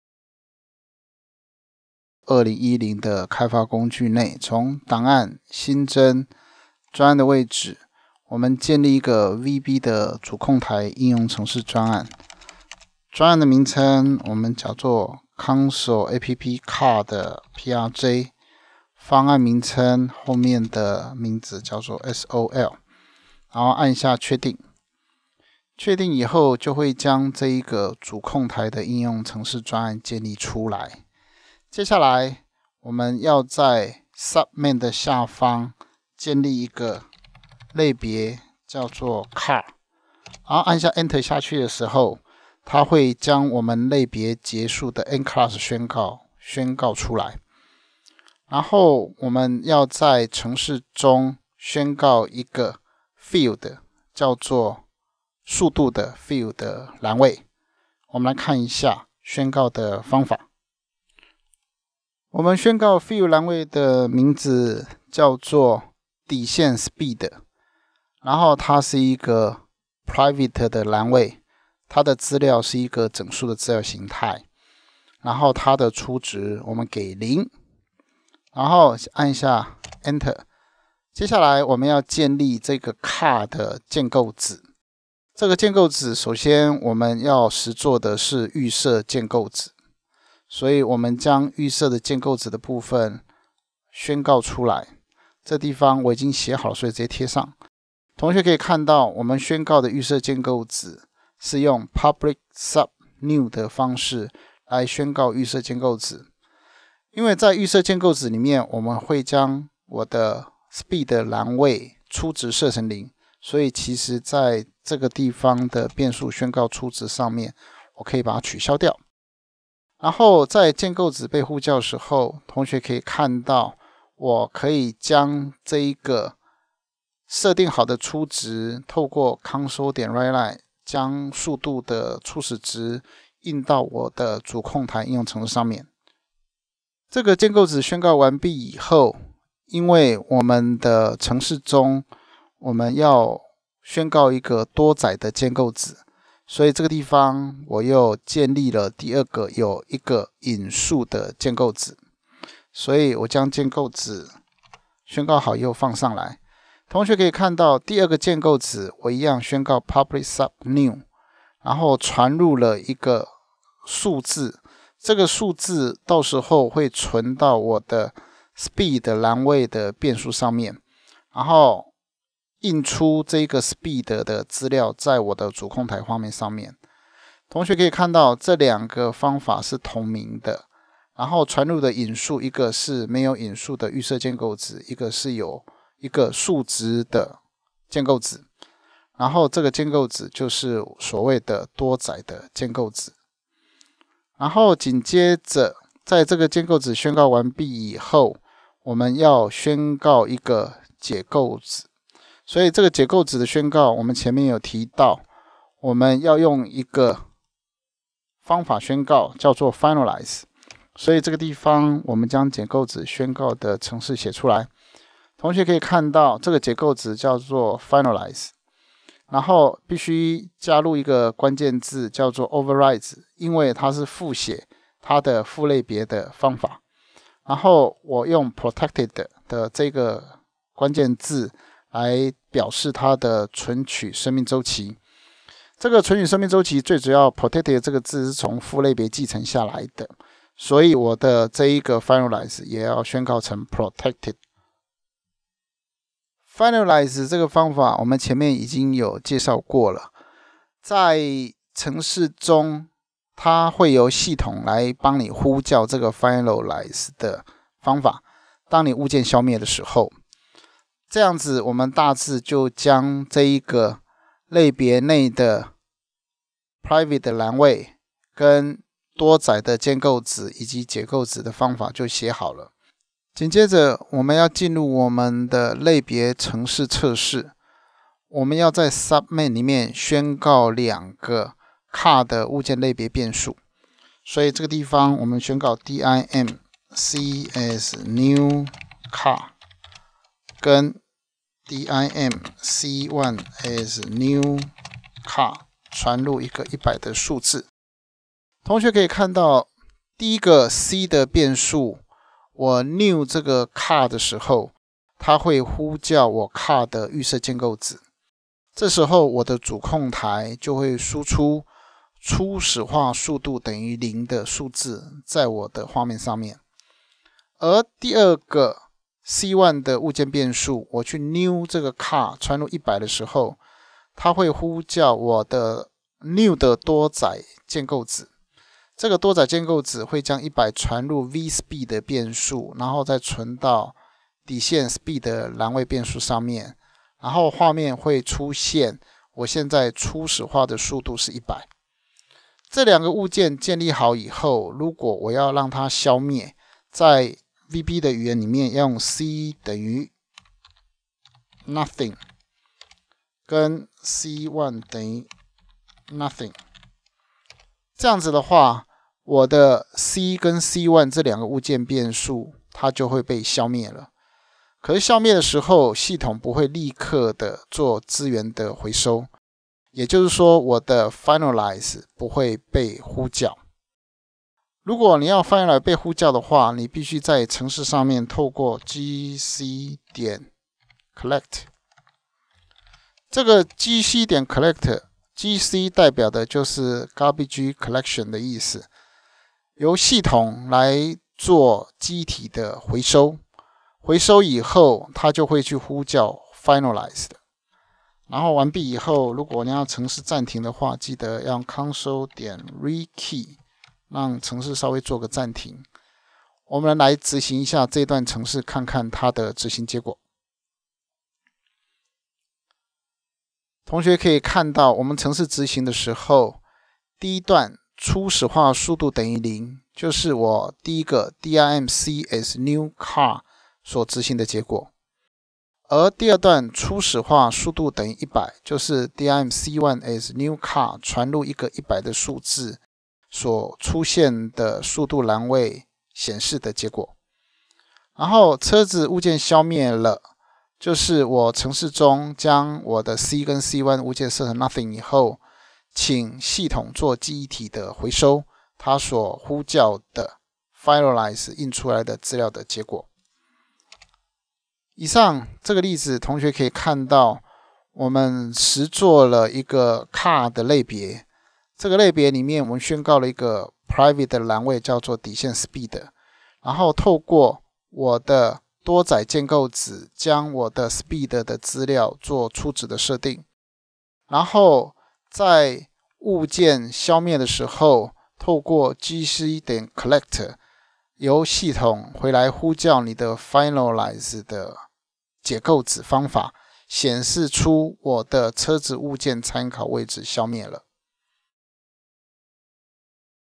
VB.NET-物件導向語言程式設計
微軟Visual Studio 2010開發工具，可在最新的微軟作業平台上快速建立應用程式。新技術可以讓開發人員建立高效率、互動式及企業網站應用程式。目前各大企業已經大量利用.NET平台開發所需的商業應用程式，學習本課程可以讓學員了解Visual Studio 2010開發工具並透過使用VB.NET語言，熟悉所需具備基本物件導向程式語言基礎，並進而獲得相關認證的考試所需具備的基礎知識。